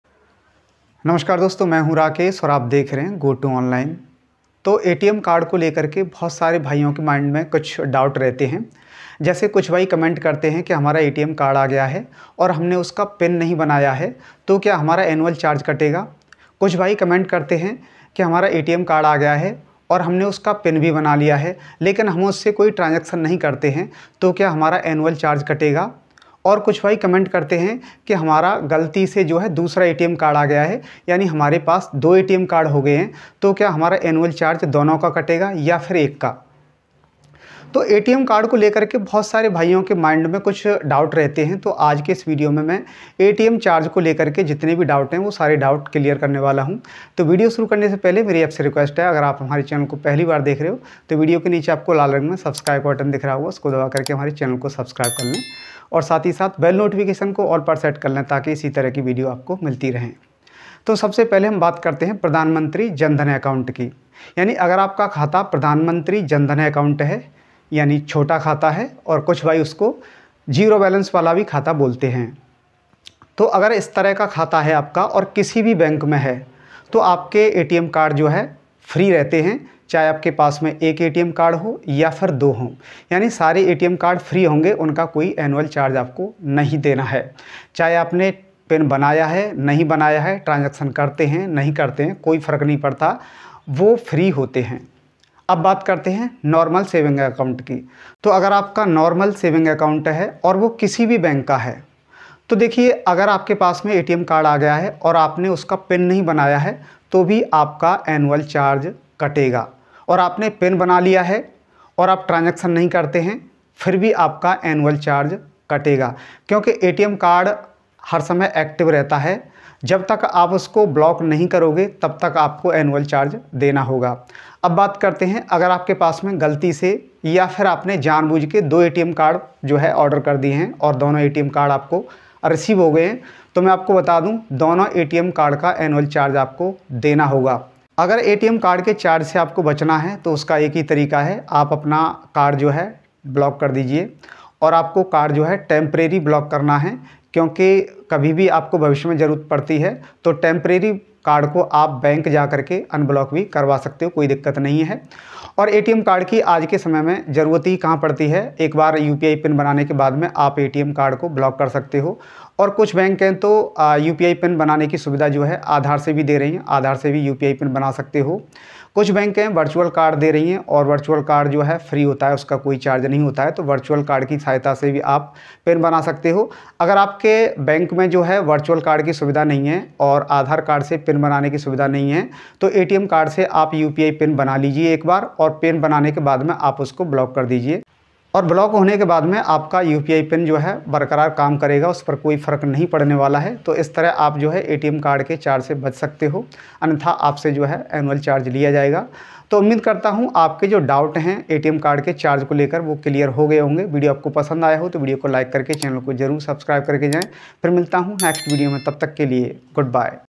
ATM का पिन नहीं बनाया चार्ज कटेगा या नहीं? || Atm use nahi kiya annual charge katega ya nahi
मैंने इस वीडियो में बताया है अगर आप एटीएम कार्ड का पिन नहीं बनाते हो तो क्या आपको एटीएम चार्ज देना होगा या फिर आप अपना एटीएम कार्ड अगर यूज नहीं करते हो तो क्या आपको एनुअल चार्ज देना होगा या फिर नहीं देना होगा
बिना एटीएम कार्ड यूपीआई पिन कैसे बनाएं
एटीएम कार्ड ब्लॉक करने के बाद यूपीआई पिन काम करेगा या नहीं
will work UPI pin after block atm